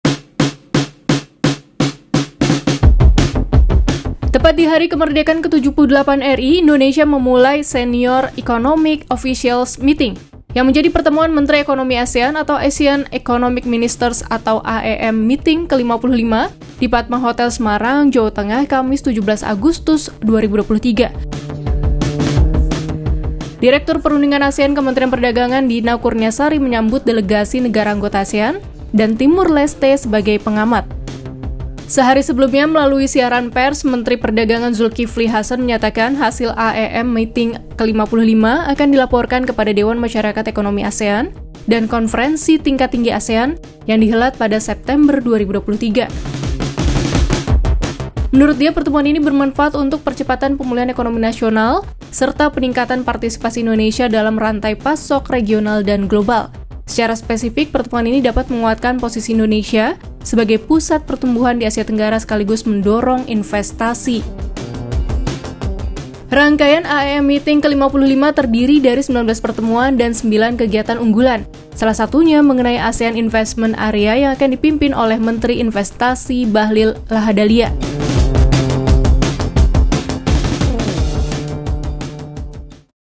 Indonesia Mulai Pertemuan Menteri Ekonomi ASEAN ke-55
Tepat di Hari Kemerdekaan ke-78 RI, Indonesia memulai Senior Economic Officials Meeting yang menjadi pertemuan Menteri Ekonomi ASEAN atau ASEAN Economic Ministers (AEM Meeting) ke-55 di Padma Hotel, Semarang, Jawa Tengah, Kamis (17/8/2023). Direktur Perundingan ASEAN Kementerian Perdagangan Dina Kurniasari menyambut delegasi negara anggota ASEAN dan Timor Leste sebagai pengamat.

Sehari sebelumnya, melalui siaran pers, Menteri Perdagangan Zulkifli Hasan menyatakan, hasil AEM Meeting ke-55 akan dilaporkan kepada Dewan Masyarakat Ekonomi ASEAN dan Konferensi Tingkat Tinggi ASEAN yang dihelat pada September 2023. Menurut dia, pertemuan ini bermanfaat untuk percepatan pemulihan ekonomi nasional serta peningkatan partisipasi Indonesia dalam rantai pasok regional dan global. Secara spesifik, pertemuan ini dapat menguatkan posisi Indonesia sebagai pusat pertumbuhan di Asia Tenggara sekaligus mendorong investasi.
Rangkaian AEM Meeting ke-55 terdiri dari 19 pertemuan dan 9 kegiatan unggulan. Salah satunya mengenai ASEAN Investment Area yang akan dipimpin oleh Menteri Investasi Bahlil Lahadalia.